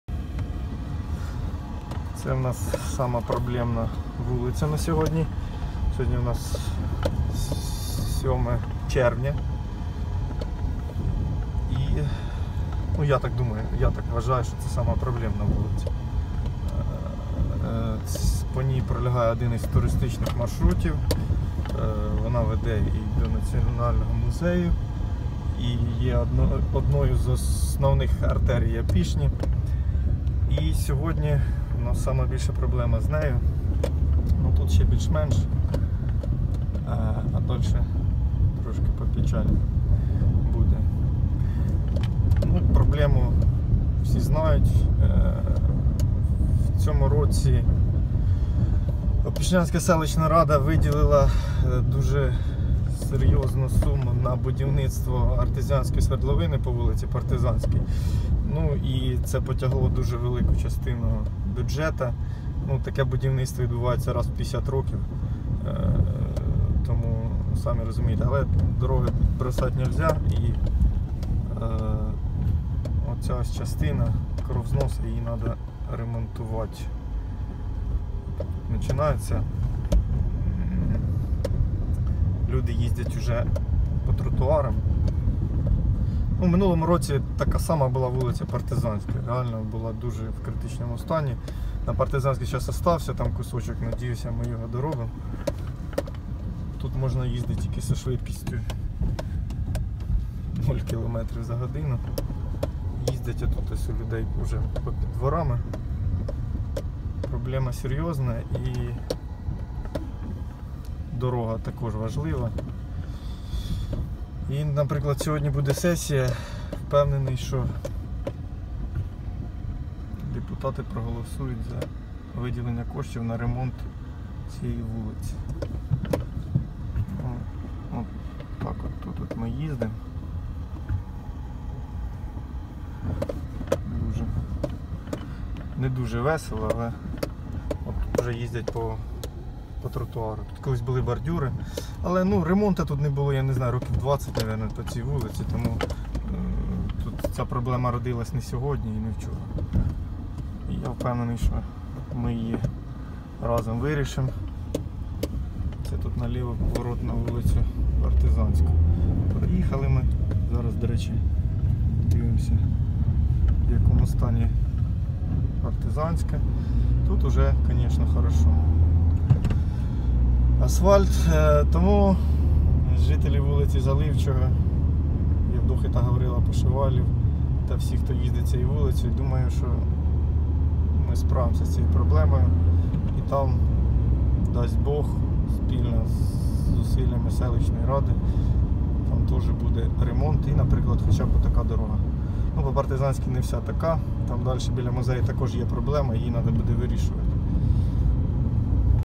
Стан дороги вулиця Заливчого (Опішня 07.06.18)
Вулиця, яка потребує негайного ремонту